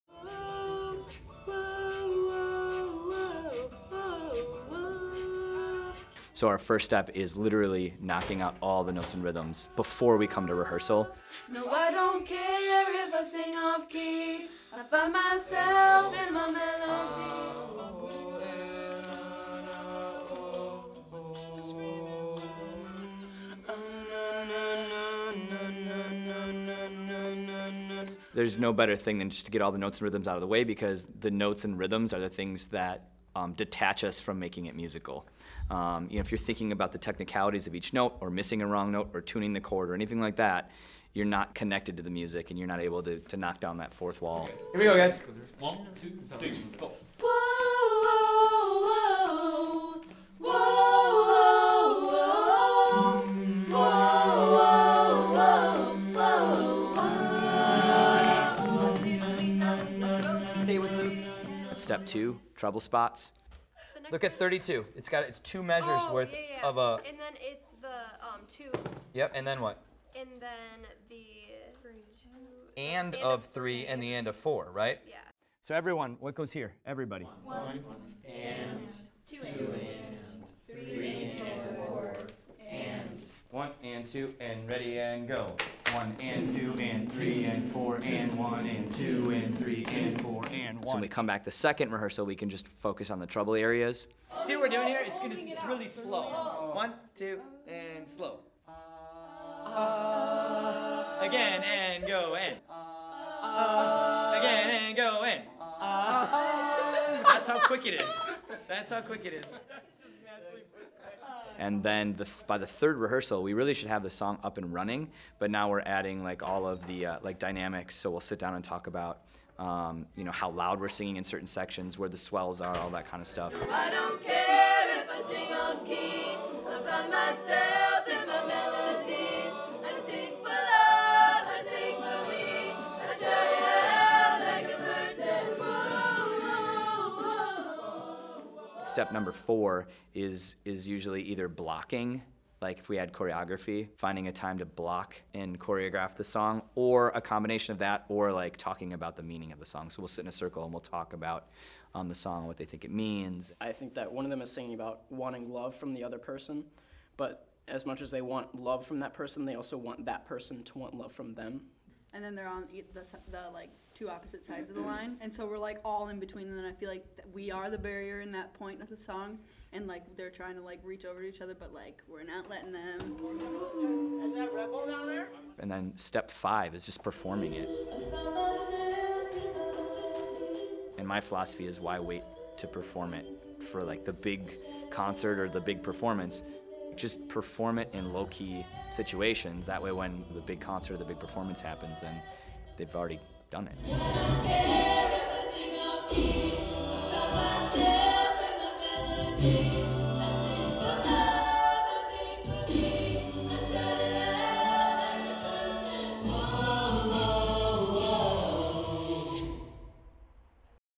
G.E.T. Vocal Point - Rehearsal Process
GET choral director Ryan Stuempges and the students of G.E.T. Vocal Point show how they prepare their a cappella performances using a five step rehearsal process.